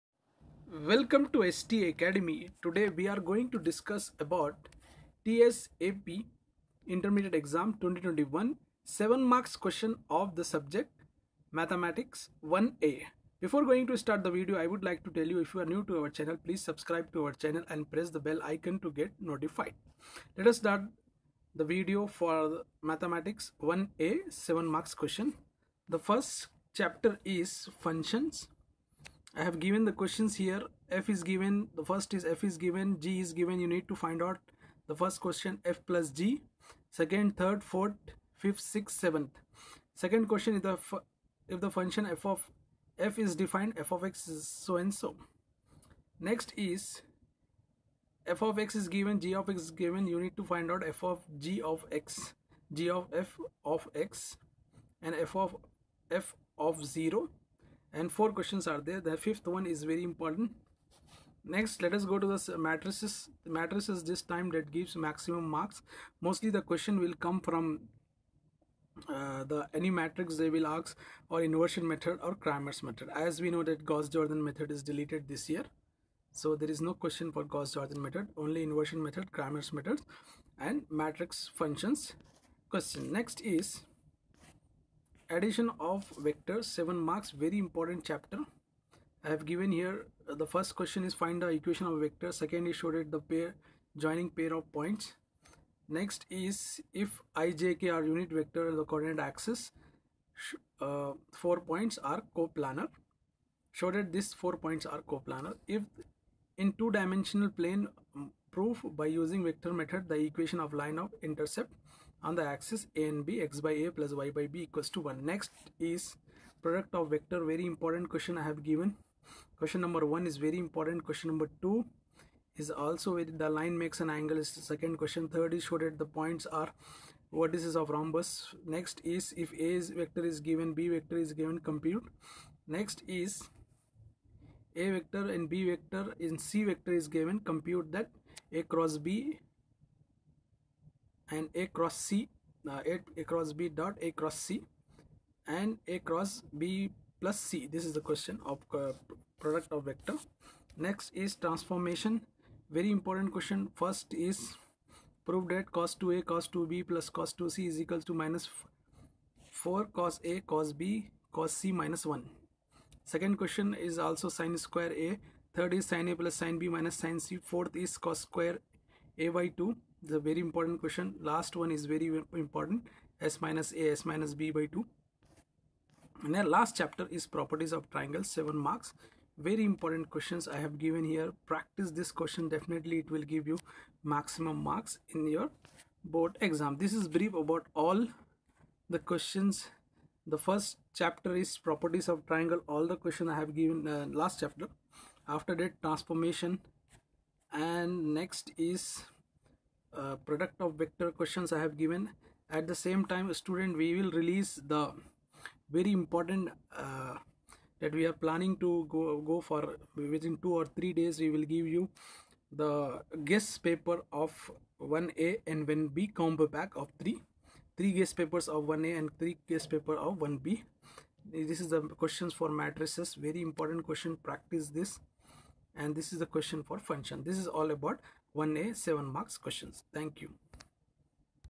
TS/AP Intermediate Exam 2021|Math 1A Inter 1st Year 7 Marks Top LAQ according to new paper pattern
Click the link for blue print

click here for SAQ

click here for vsaq